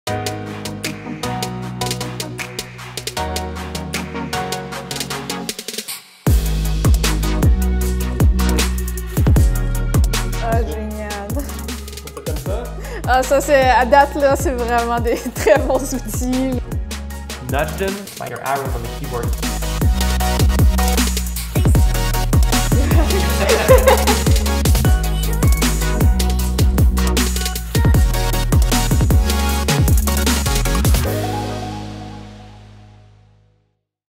Harmony 16 Shift and Trace and Deformer Manipulations Features Reactions
Through a set of powerful new tools, Harmony 16 supports pipeline integration and helps you be more efficient, increase your productivity and solidify your workflow process. These lucky animators learned how to modify multiple points simultaneously and discovered Harmony 16's new Shift and Trace tools before the launch — watch their reactions!

Special thanks to Oasis Animation